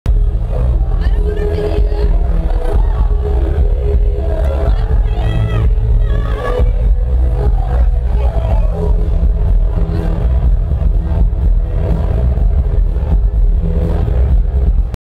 Propaganda - What is your perfect night out?
O2 asked fans at O2 Academy Birmingham: "What is your perfect night out?"